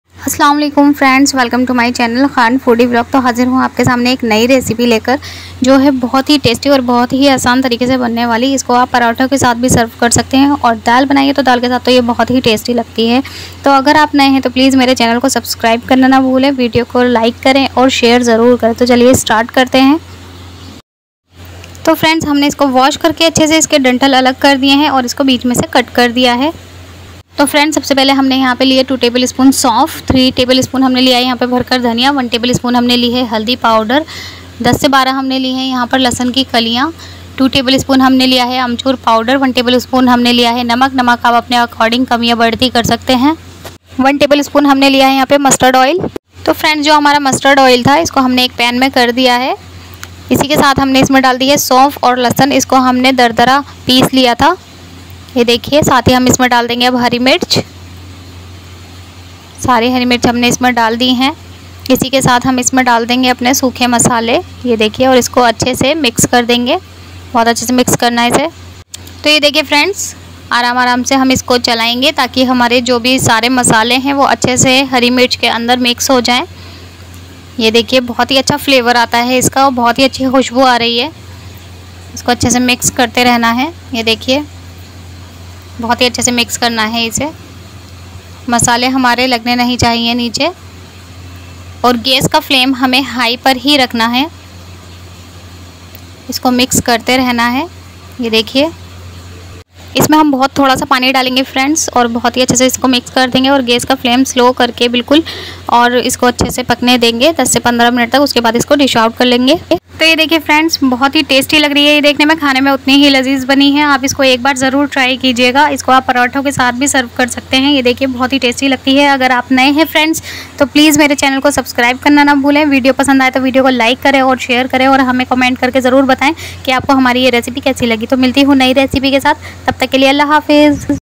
jhatpat bnayen hari mirch msala ll bahut hi testy recipe ll bahut hi yummy.
250 gram hari mirch

3 chammach dhaniya powder
2 chammach souf
2 chammach amchur
1 chammach namak
1 chammach haldi
8 se 10 lehsun ki kaliyan